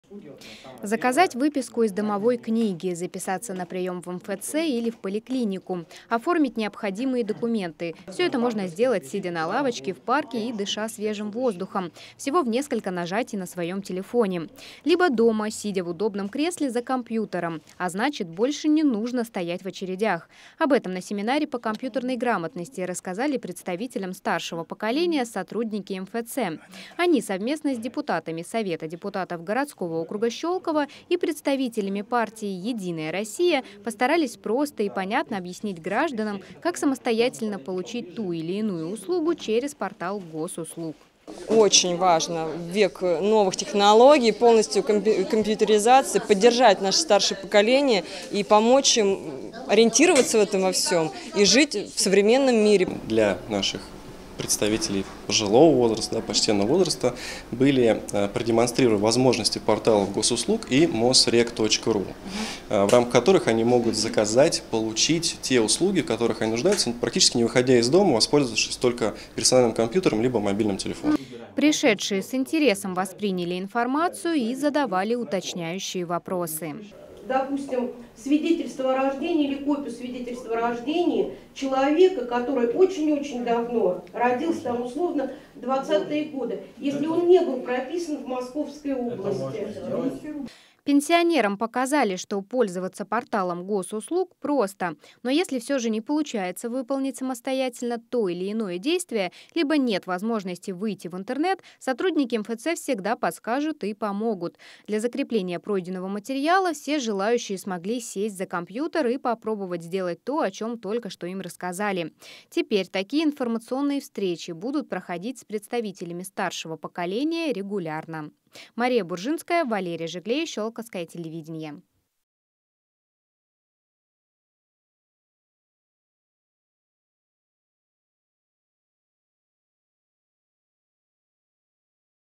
Сотрудники МФЦ рассказали щелковским пенсионерам, как пользоваться порталом «Госуслуги»
Для щёлковских пенсионеров провели урок компьютерной грамотности, на котором им рассказали, как самостоятельно пользоваться порталом Госуслуг, без труда совершая там необходимые действия. Все оказалось довольно просто и удобно. Подробности в нашем следующем сюжете.